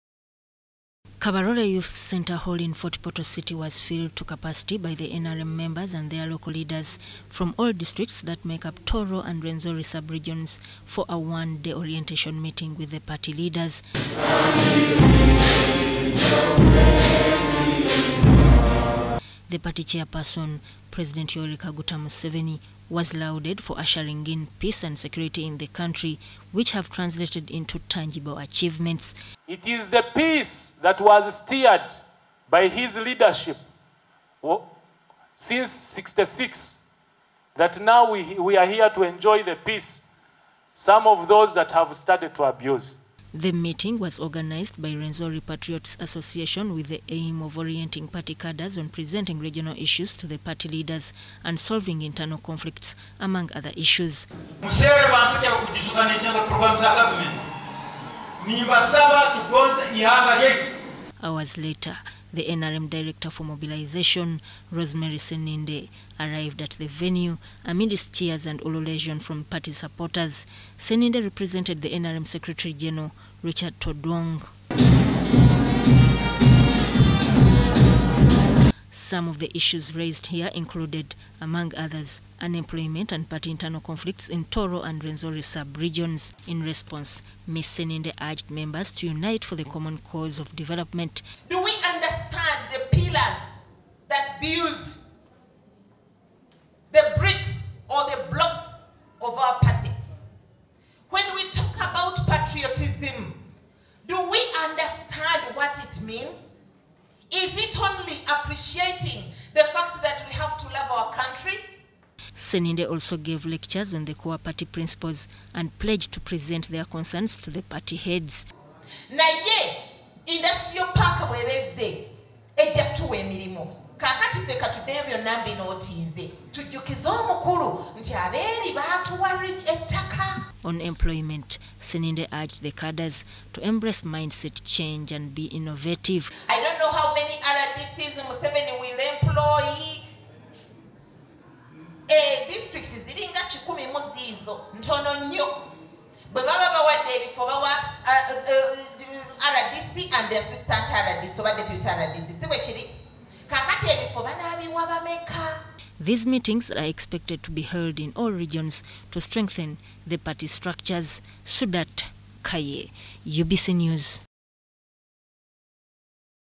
Party cohesion - NRM Party leaders meet in Fort Portal to strengthen Party structures
NRM party leaders have embarked on orientating its members on the party core principles, this, in a bid to strengthen Party structures at the grassroots.
The move according the party leaders is to present regional issues for government intervention and to address the internal conflicts in the party.